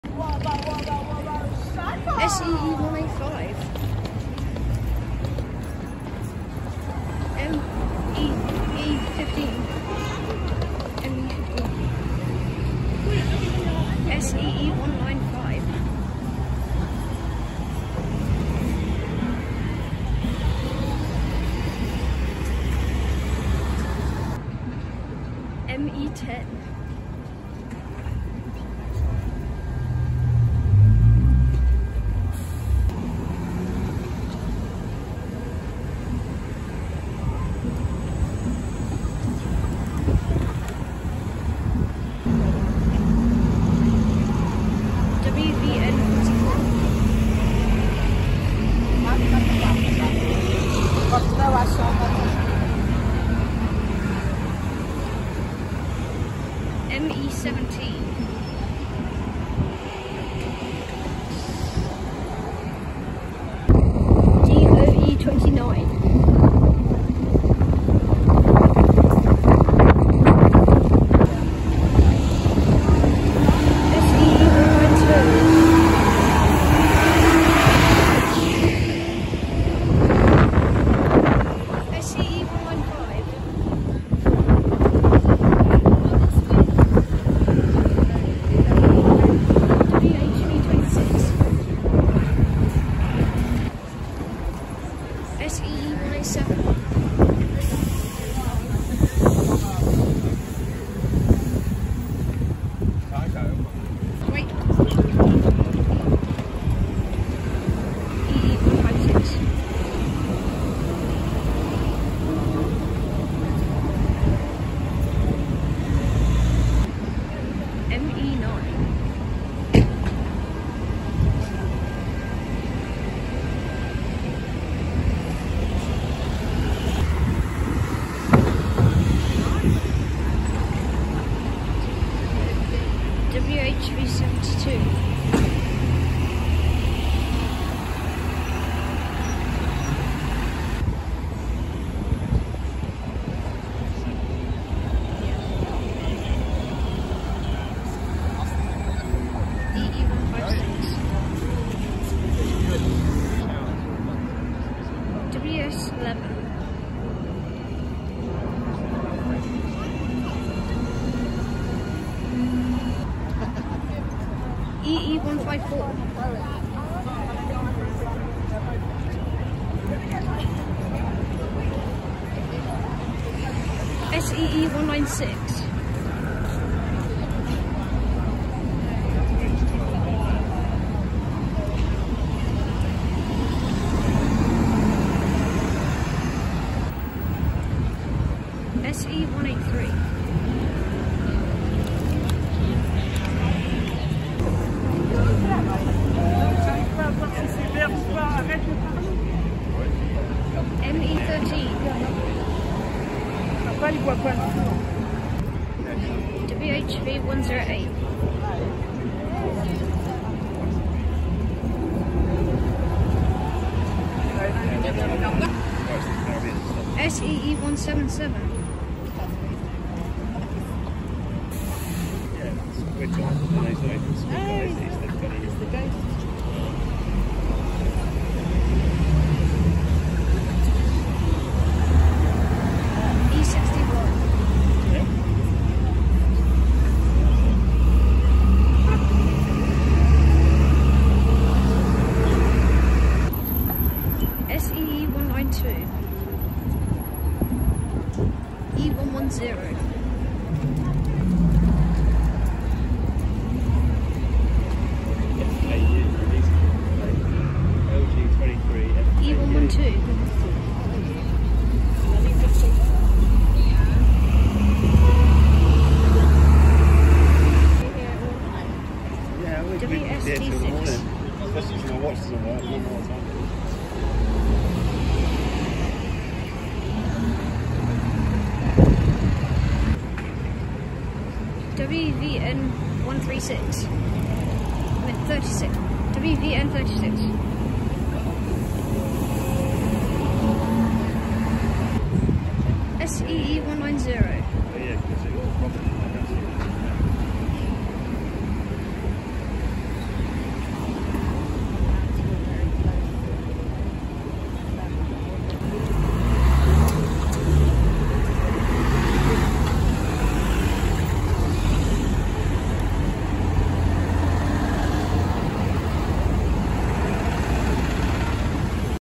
Buses in Wimbledon May 2023
This part of Wimbledon is next to the train station.

Also they’ve got some Brand New 2023 Electric Buses in Wimbledon. There are BYD ADL Enviro200EV’s in routes 163 and 164 and BYD ADL Enviro400EV’s in route 93. They are Go-Ahead London Buses.